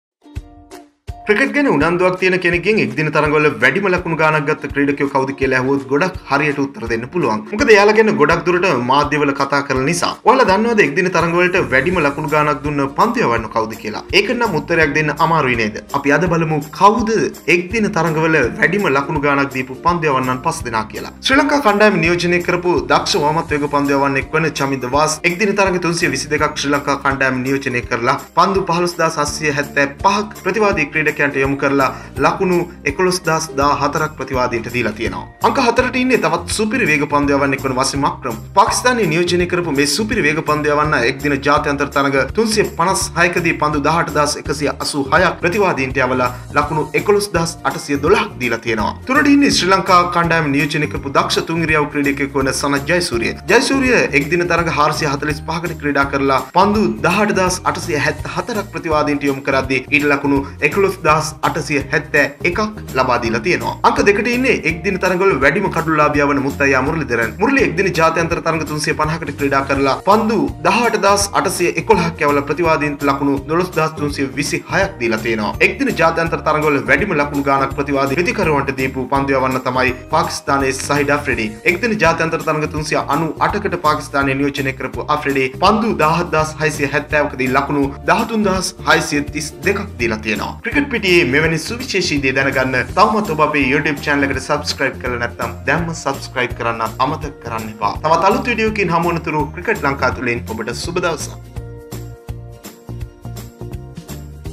එක්දින තරඟ වල වැඩිම ලකුණු දුන් මුල් පහෙන් තුනක්ම අපේ
ක්‍රිකට් ගැන උනන්දුවක් තියෙන කෙනෙක්ගෙන් එක්දින තරගවල වැඩිම ලකුණු ගාණක් ගත්ත ක්‍රීඩකයෝ කව්ද කියලා ඇහුවොත් ගොඩක් හරියට උත්තර දෙන්න පුළුවන්.

මොකද එයාල ගැන ගොඩක් දුරට මාධ්‍ය වල කතා කරන නිසා ඔයාල දන්නවද එක්දින තරගවල වැඩිම ලකුණු ගාණක් දුන්න පන්දු යවන්නෝ කවුද කියලා .

ඒකට නම් උත්තරයක් දෙන්න අමාරුයි නේද අපි අද බලමු කවුද දින තරගවල වැඩිම ලකුණු ගාණක් දීපු පන්දු යවන්නන් පස්දෙනා කියලා